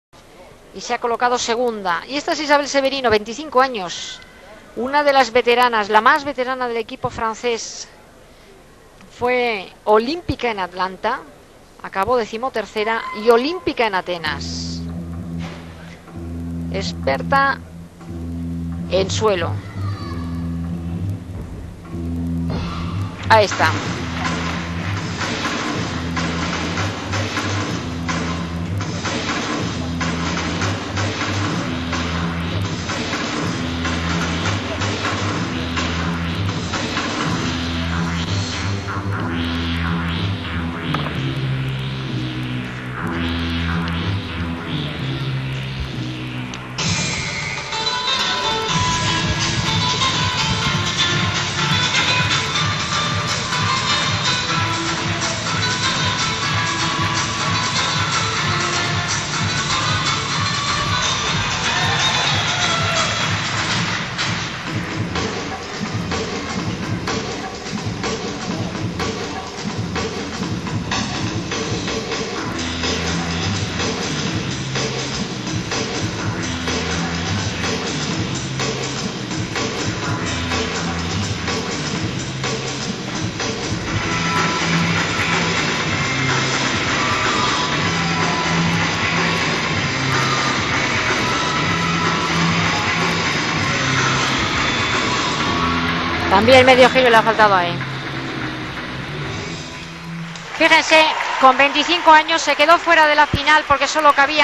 Isabelle Severino 2005 Euros EF FX
Isabelle Severino FRA
2005 Europeans, event finals - floor exercise
9.575 1st